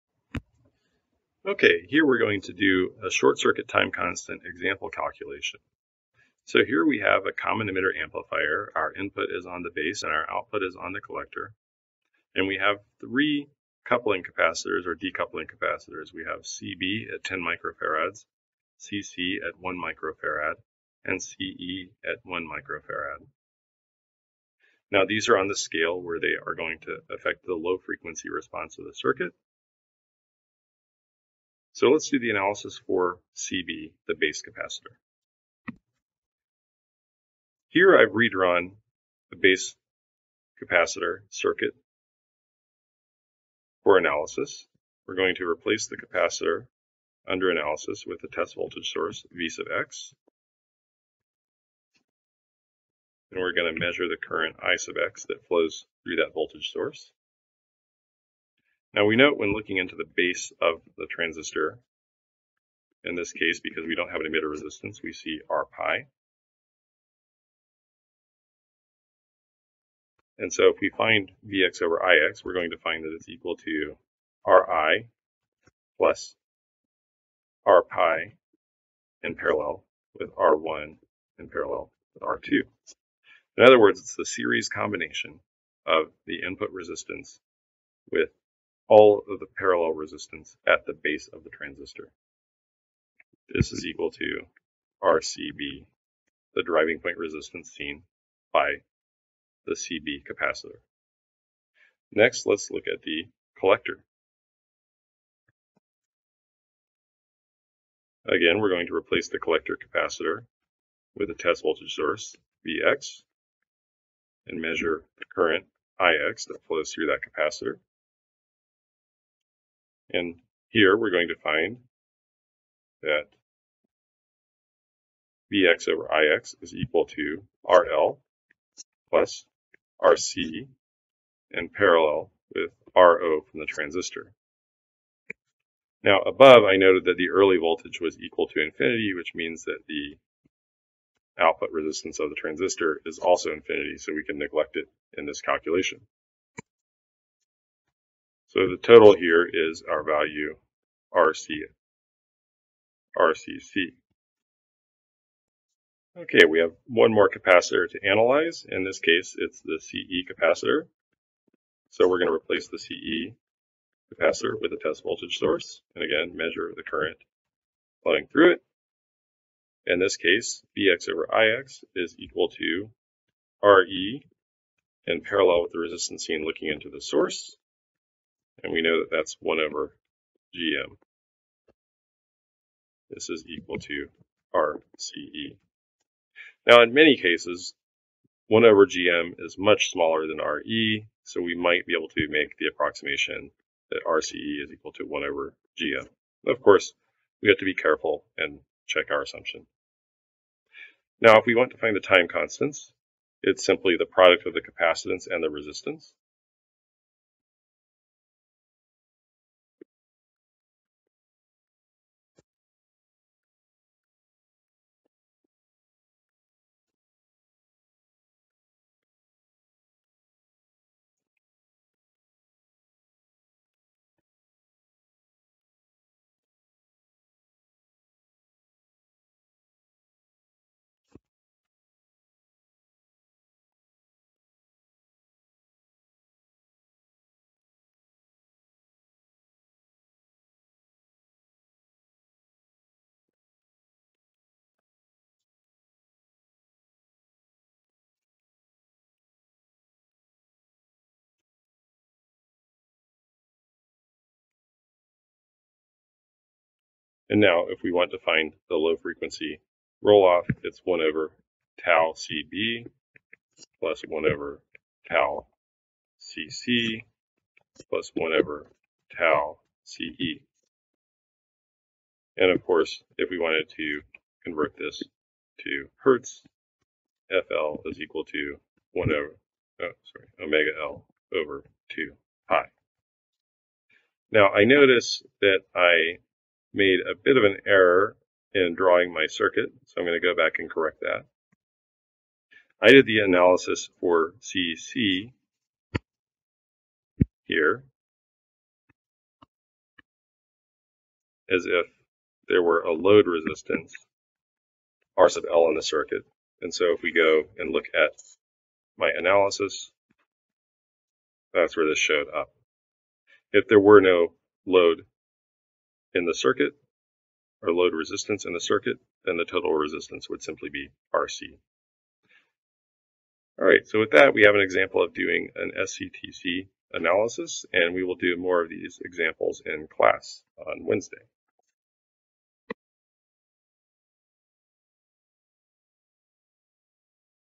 ECE 3204 Lecture 8B - SCTC Example Calculation
In this video, we do an example calculation using SCTC to find the low frequency response of an amplifier circuit.